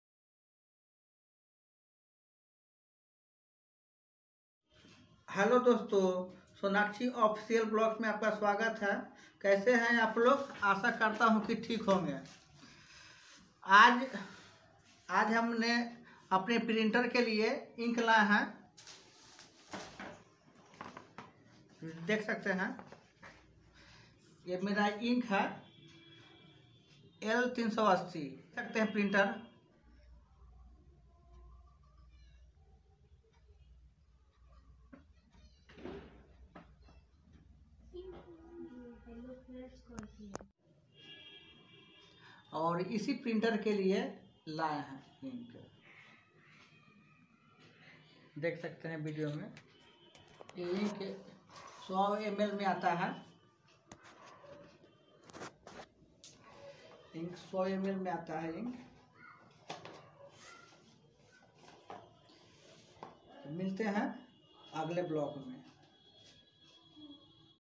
printer ke liey ink lay
welcome to sonakshi officoal vlog 20
Aapne printer ke liey ink lay or
video banaya, video pasand aaya
Support kare